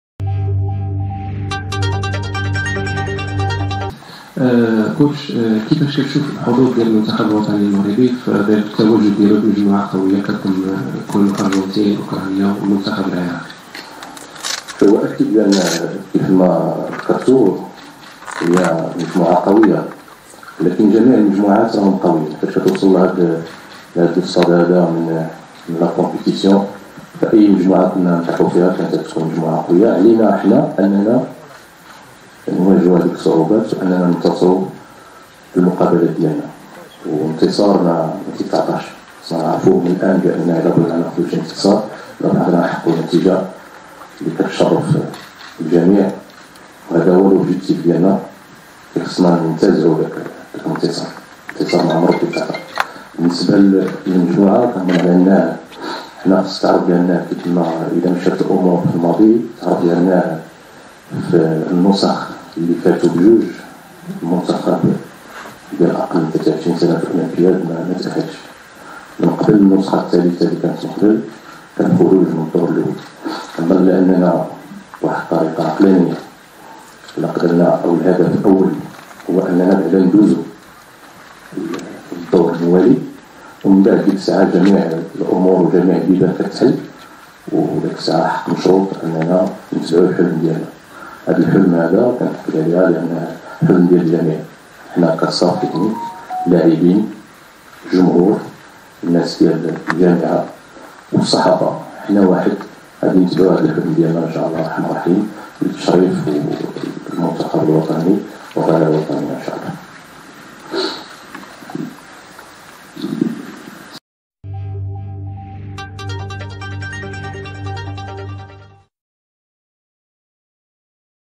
السكتيوي مدرب المنتخب الأولمبي:جميع المجموعات قوية وحنا ما علينا إلا نواجهو الصعوبات
كشف السكيتيوي في ندوته الصحفية تفاصيل مثيرة قبل الذهاب لأولمبياد باريس.